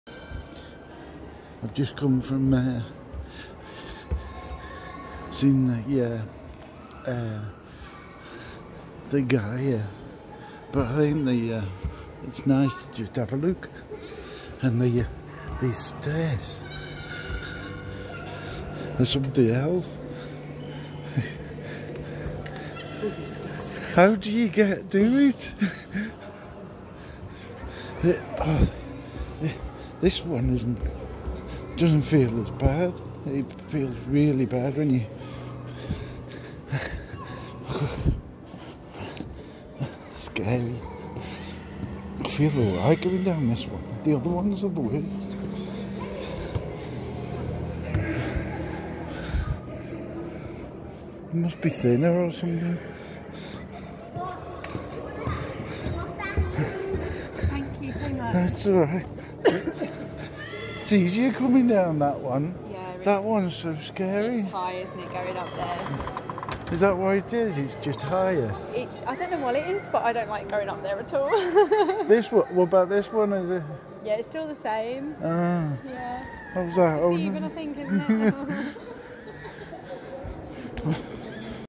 Liverpool cathedral gaia
A walk down from seeing platform so scary but why